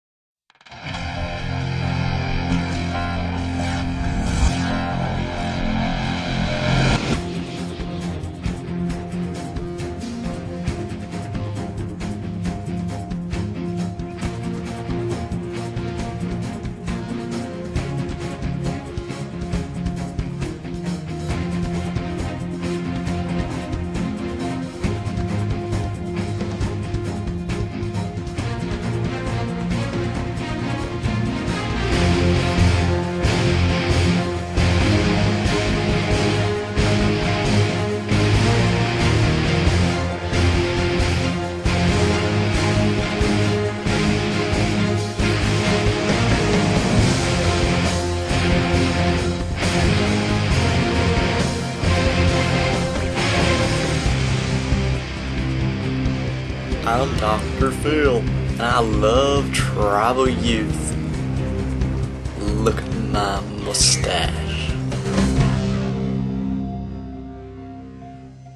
T.R.I.B.A.L Youth Promo - 2009
Promotional video for T.R.I.B.A.L YOUTH...a ministry of the Bowen Baptist Church (2009)